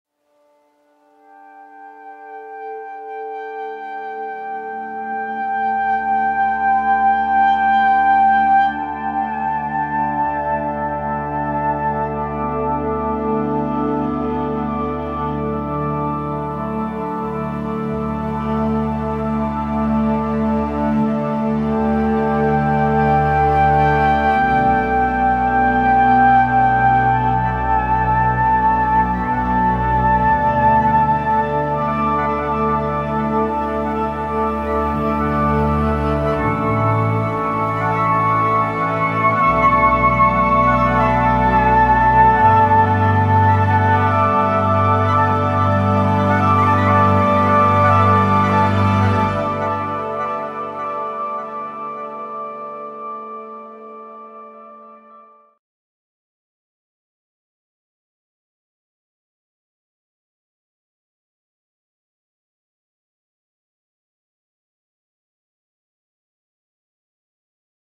Trailer: Clarinet Textures by Emergence Audio - Free Kontakt Player Ready and NKS Compatible
Introducing Clarinet Textures by Emergence Audio, a powerful new way to create evolving pads, soundscapes, drones, and pulses that never feel static. Add profound expression to your music. Powered by the Free Kontakt Player 6.7+ and Fully NKS compatible